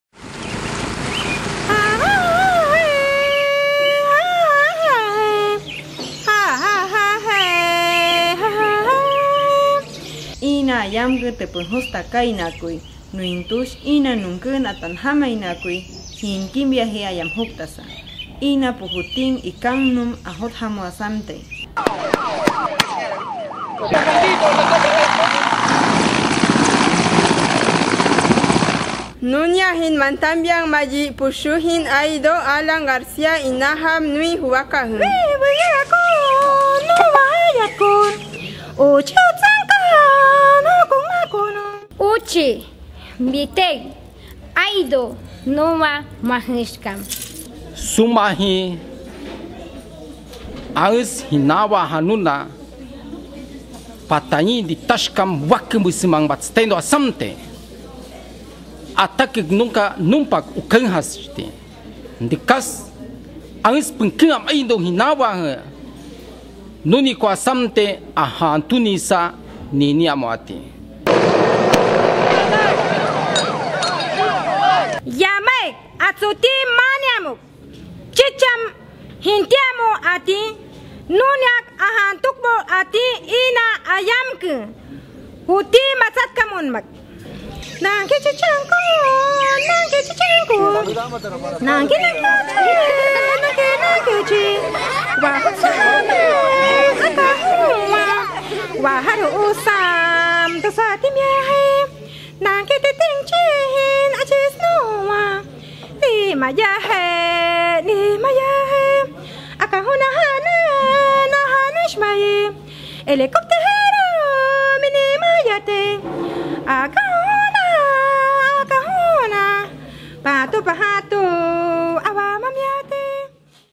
Mensaje Awajún a dos años del Baguazo
A dos años de los luctuosos sucesos de Bagua, dos videos resumen el sentimiento y la postura de los pueblos awajún y wampís. El video fue producido en el marco del Taller de Comunicación para el fortalecimiento de la Organización Regional de los Pueblos Indígenas de la Amazonía Norte (ORPIAN-P), realizado del 29 de mayo al 01 de junio, en la ciudad de Bagua Chica. Más información en nuestra nota web